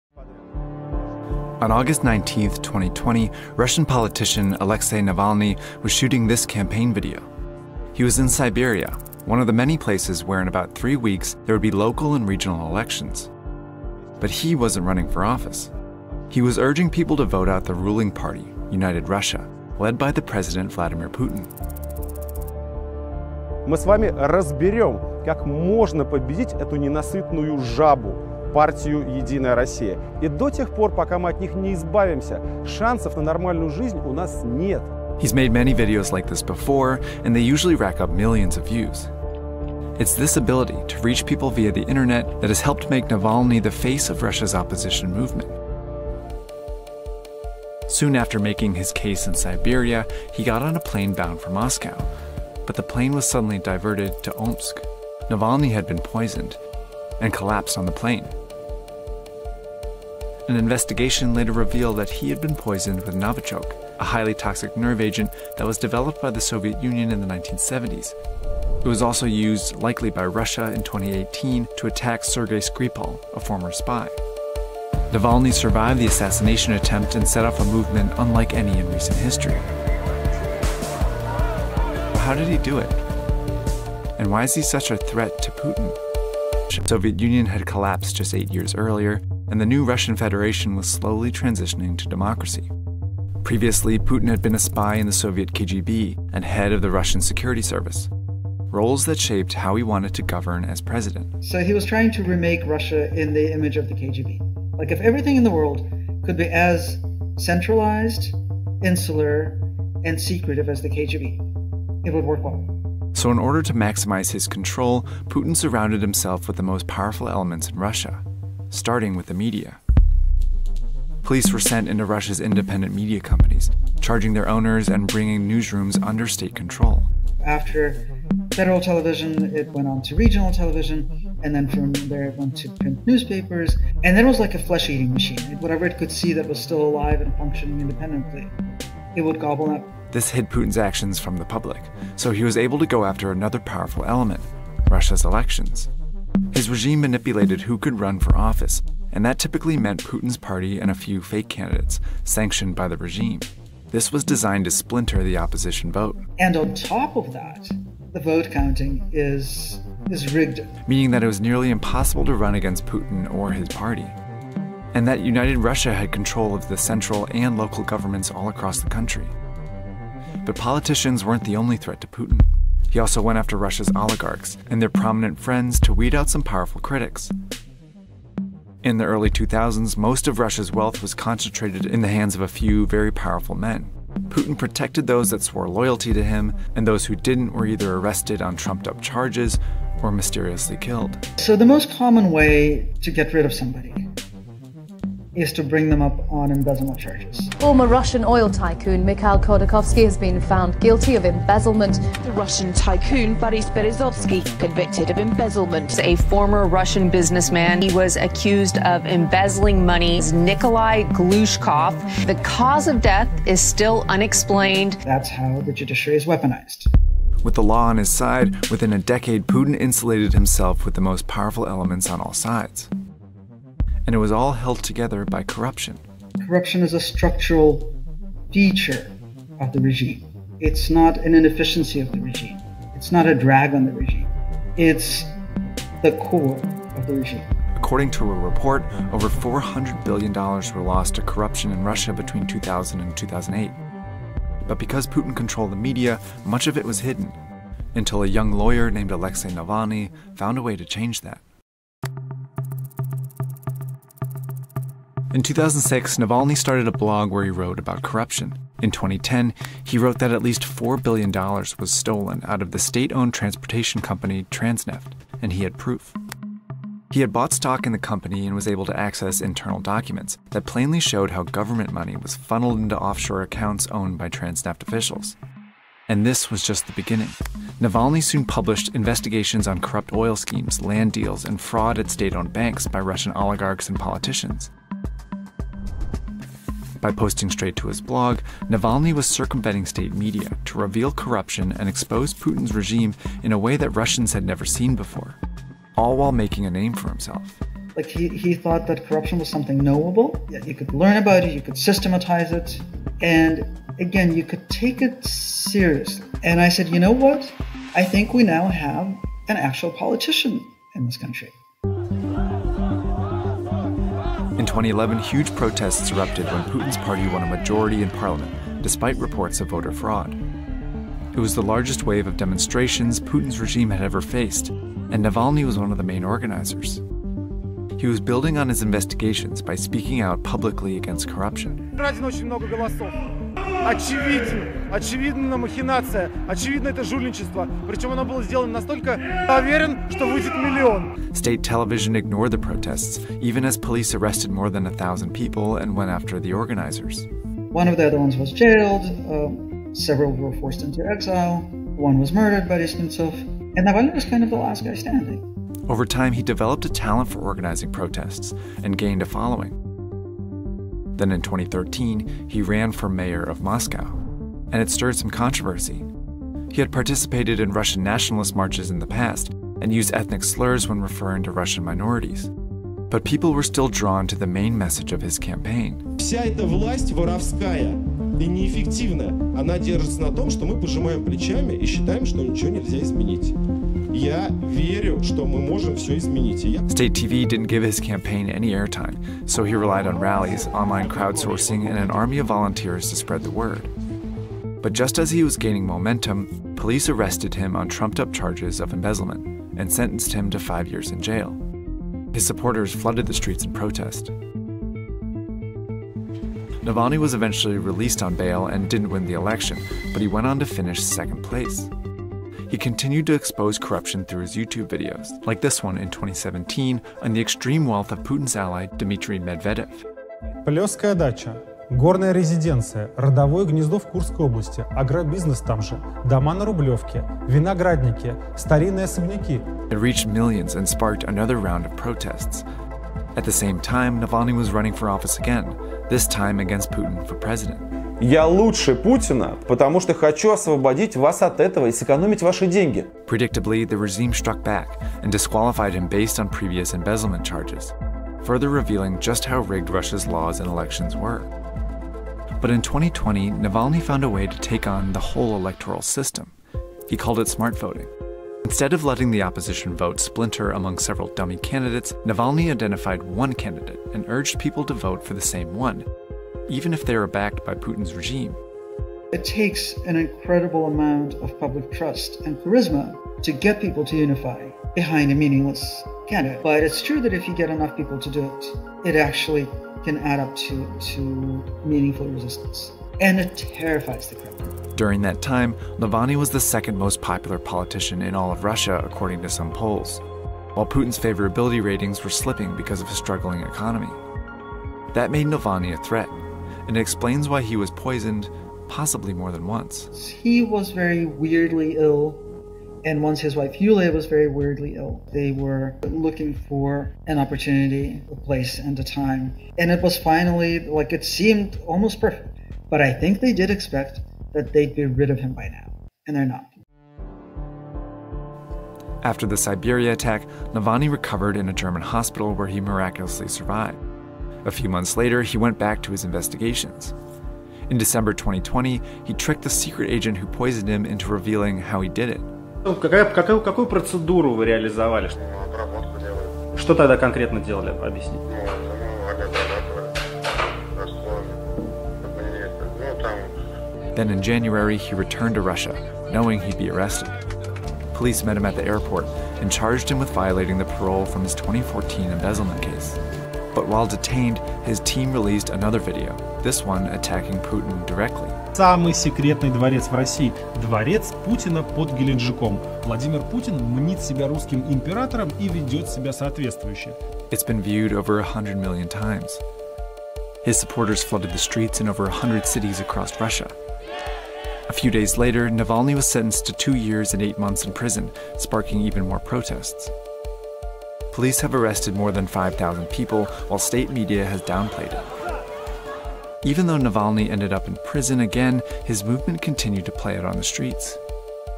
Why Putin wants Alexei Navalny dead
Navalny's movement is unlike any in recent history.

In 2006, a lawyer named Alexei Navalny started a
blog where he wrote about corruption in his home
country of Russia. It's the most prominent problem
under the regime of Vladimir Putin, who has ruled
Russia since 2000. Putin has systematically taken
over the country's independent media, oligarchy,
elections, and laws to cement his own power and
wield corruption to his advantage.